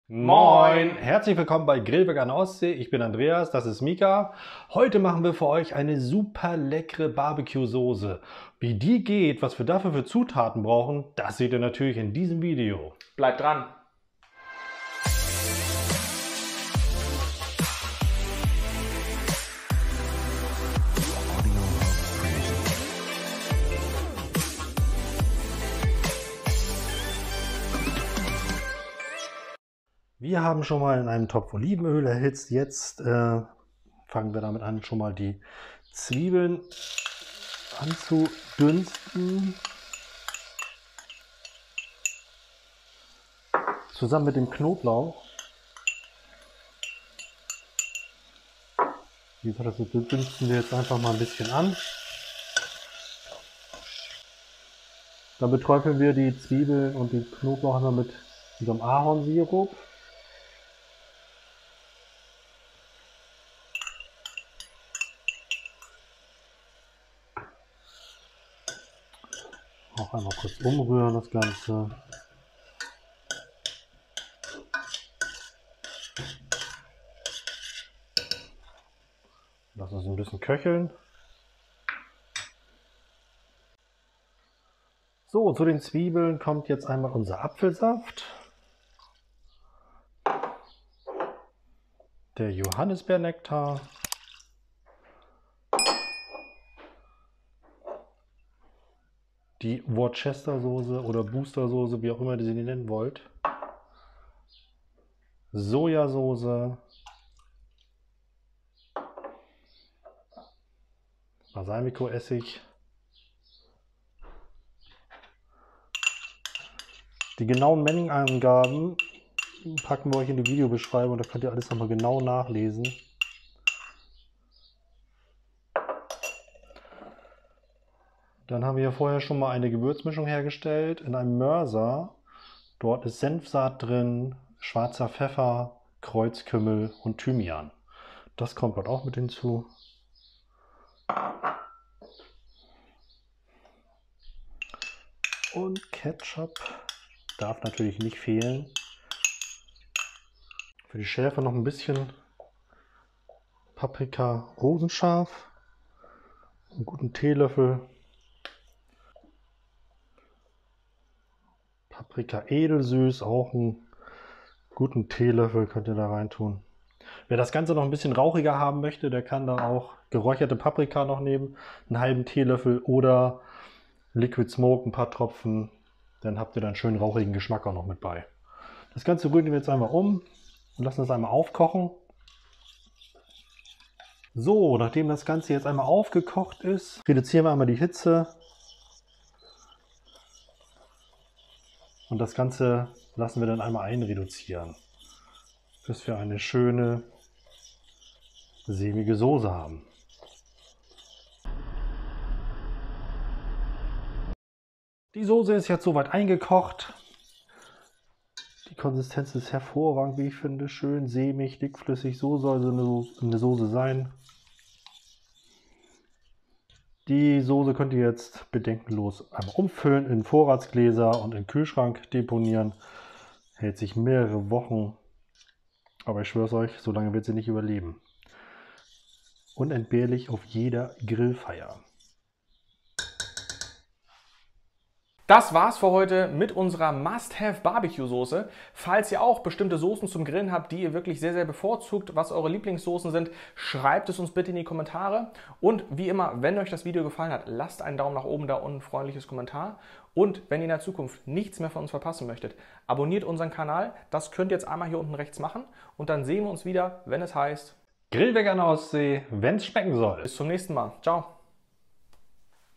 BBQ Sauce selber machen - einfach und genial 🤩
Wir zeigen euch heute, wie euch eine leckere BBQ Sauce ohne großen Aufwand gelingt. 😋

Zutaten für ca. 300 ml:
1 TL Kreuzkümmel (Cumin)
1 EL Senfsaat
1/2 TL schwarzer Pfeffer
1 TL Thymian
1 TL Paprikapulver (rosenscharf)
1 TL Paprikapulver (edelsüß)
120 g Zwiebeln
1 frische Knoblauchzehe
30 ml Olivenöl
20 ml Ahornsirup
100 ml schwarzer Johannisbeernektar
100 ml Apfelsaft
100 ml Ketchup
40 ml Sojasauce
30 ml Worcestershiresauce
30 ml Balsamico Essig (6% Säure)